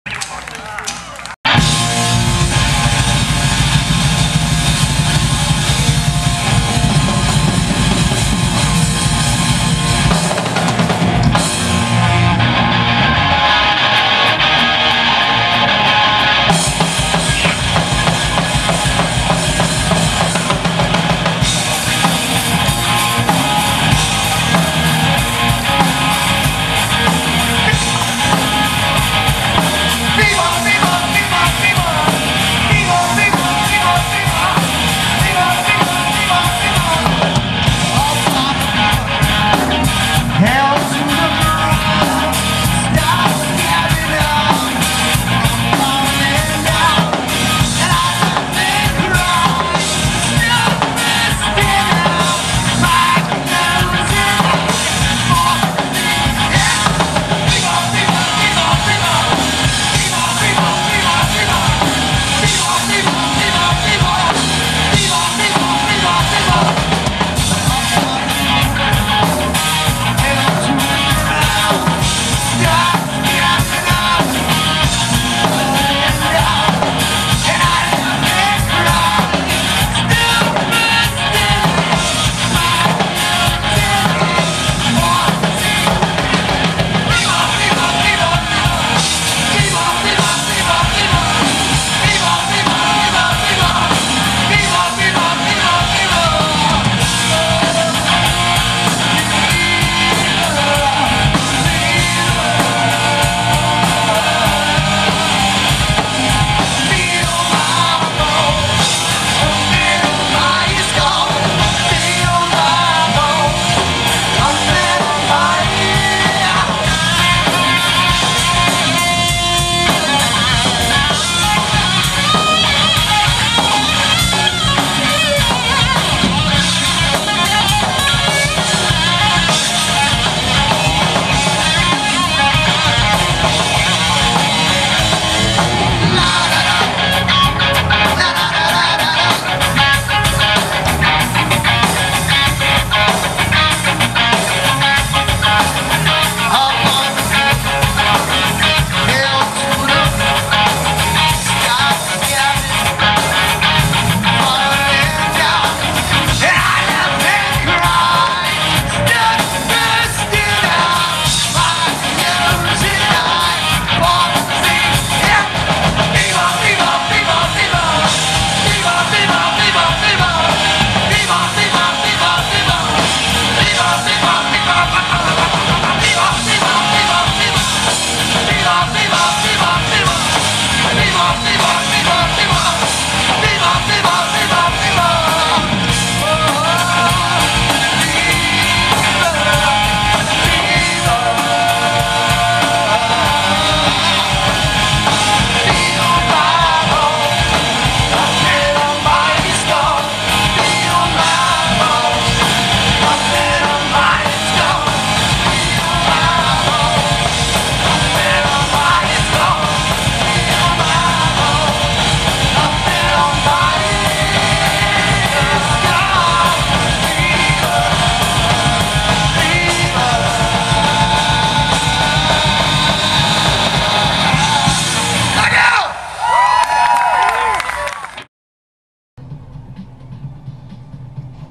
Hornit - BAMFEST 2012
Hornit at BAMFEST 2012, a memorial for Mark Bam Bam McConnell.  Recorded by Steve McMullen at Florida Rocks! in Gainesville, Fl.